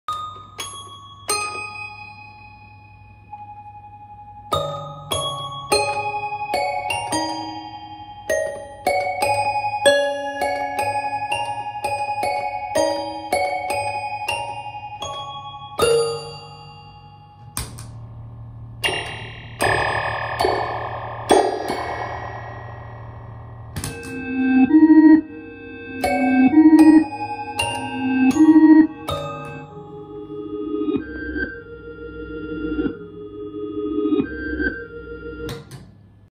Jaymar Electric Circuit Bent Modded Toy Piano Keyboard 1960's Natural
Demo with acoustic volume, full electric volume, an Outlaw Chorus guitar pedal, and a Montreal Assembly Count to Five pedal. For sale on Reverb.com by Stokkers' Musical Instruments. It can also be searched by the title of this video. Thanks!